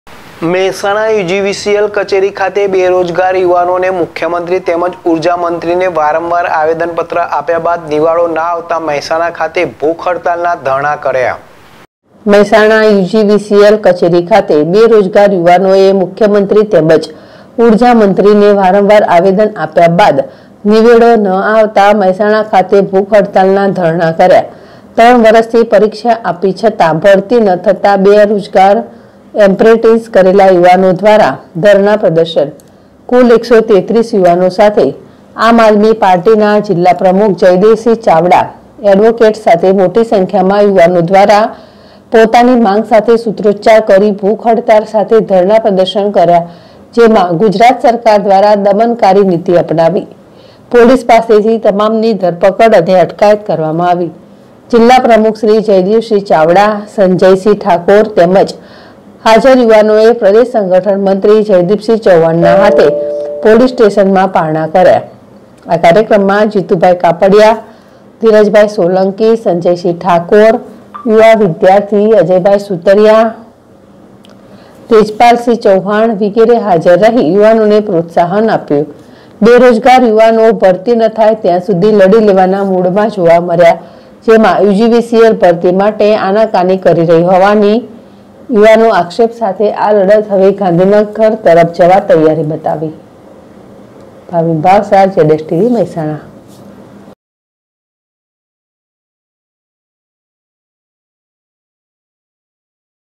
મહેસાણા યુ જી વી સી એલકચેરી ખાતે બેરોજગાર યુવાનોએ ભૂખ હડતાલ પર ઉતર્યા હતા.
મહેસાણા યુ જી વી સી એલ
કચેરી ખાતે બેરોજગાર યુવાનોએ ભૂખ હડતાલ પર ઉતર્યા હતા.